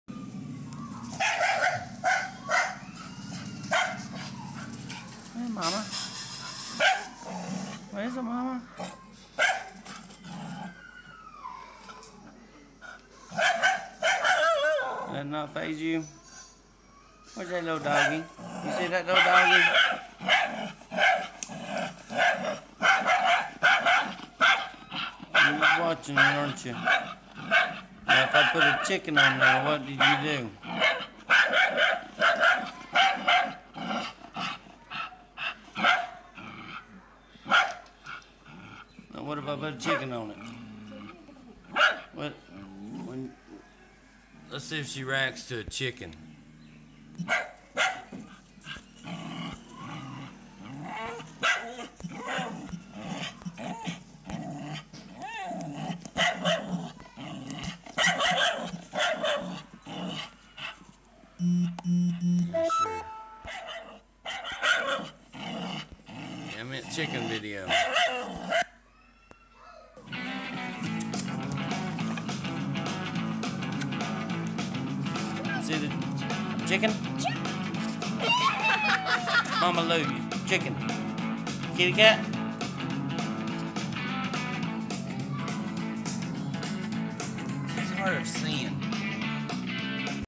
Mama Lou has hardly any reaction...
I think this dog is deaf and can only hear chicken noises, ever since she killed a chicken and a pig she has the taste for blood...Did anyone hear about Ryan Upchurch having to go to the hospital... I saw a picture and his titties reminded me of a pot belly pig suckling teets...